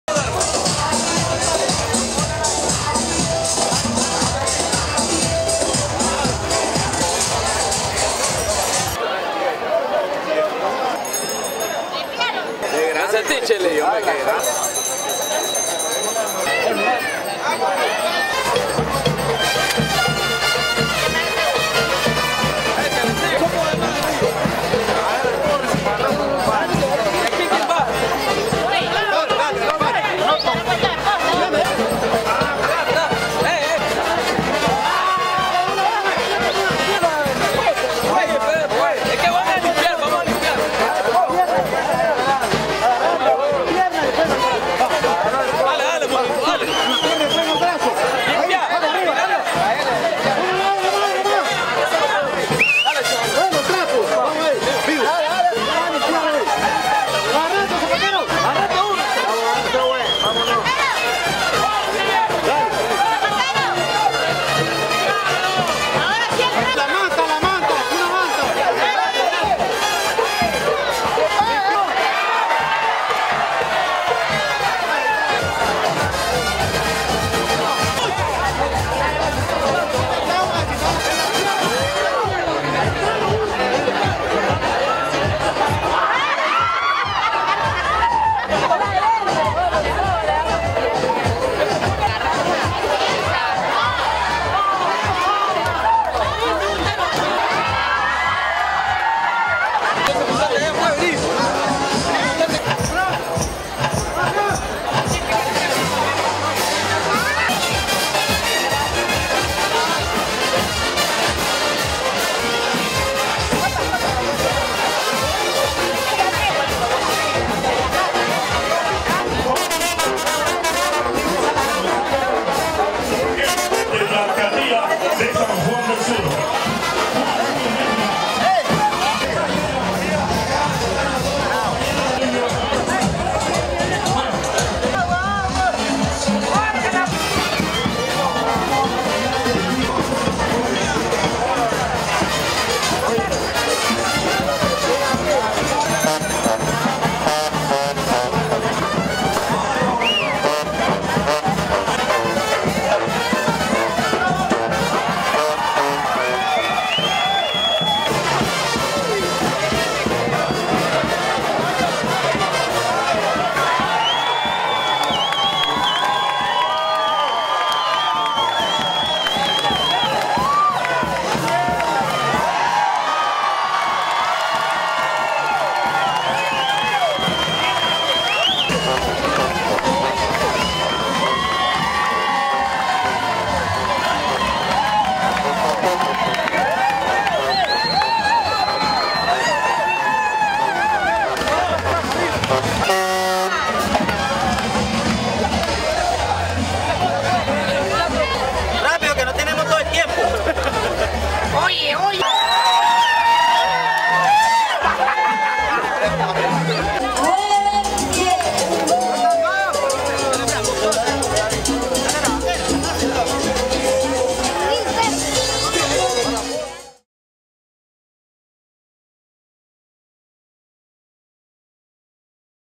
Festivales Patronales Pole climbing 2017
The greased pole is a popular part of the Patron Saint festivities in San Juan del Sur, Nicaragua